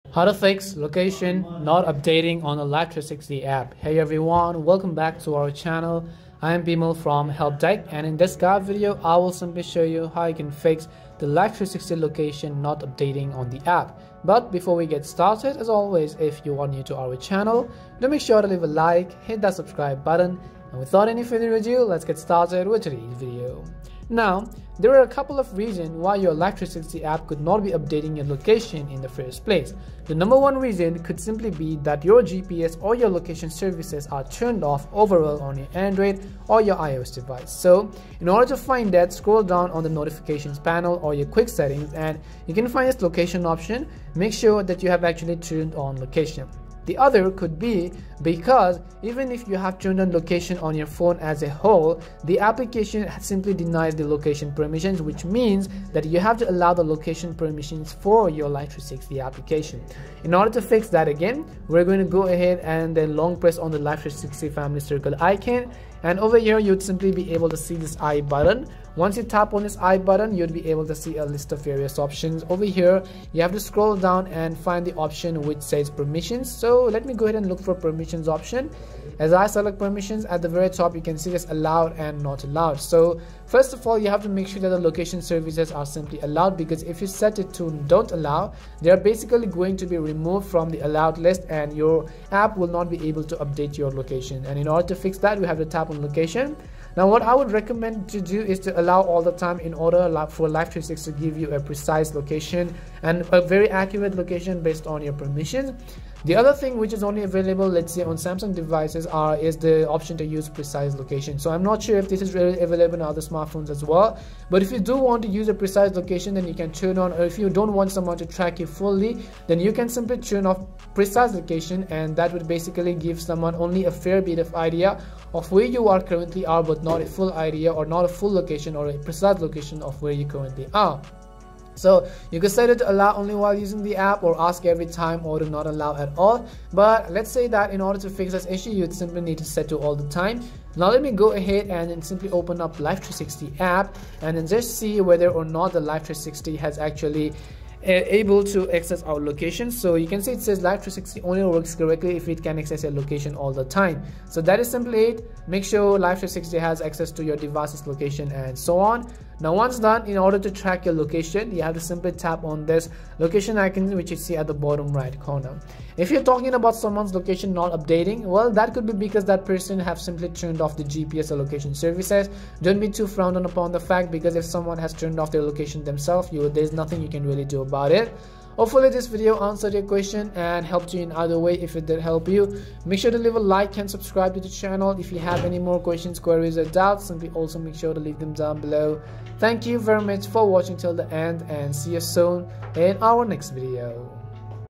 How to Fix Life360 Location Not Updating !
This video guides you in quick easy steps to fix if your Life360 does not update your location. So make sure to watch this video till the end.

1) Make sure that you have enabled location on your phone overall
2) Long press on the "i" icon
3) Go to Permissions
4) Go to Locations
5) Select Allow All the Time
6) This way, you can fix the Life360 location not updating.

0:00 Introduction
0:24 Things to Check
1:01 Fix Life360
2:58 Outro: Final Verdict